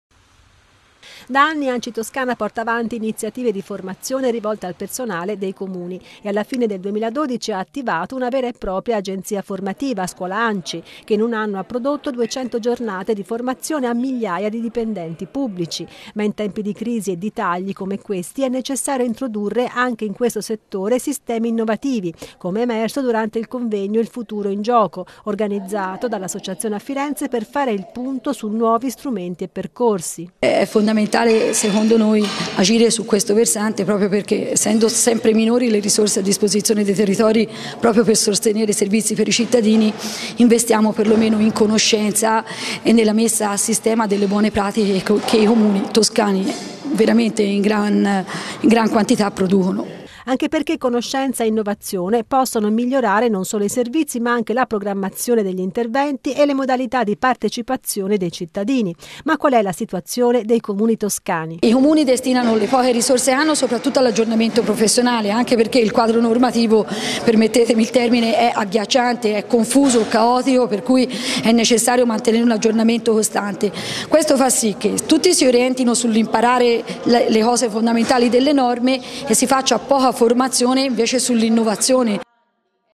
FORMAZIONE PROFESSIONALE NEI COMUNI, IL FUTURO IN GIOCO
Da anni Anci Toscana porta avanti iniziative di formazione rivolte al personale dei Comuni e alla fine del 2012 ha attivato una vera e propria agenzia formativa, Scuola anci, che in un anno  ha prodotto 200 giornate di formazione a migliaia di dipendenti pubblici.  Ma, in questi temi di crisi e di tagli alle risorse, è necessario introdurre anche in questo settore sistemi innovativi come è emerso durante il convegno ''Il futuro in gioco'' organizzato dall'associazione a Firenze per  fare il punto su nuovi strumenti e percorsi.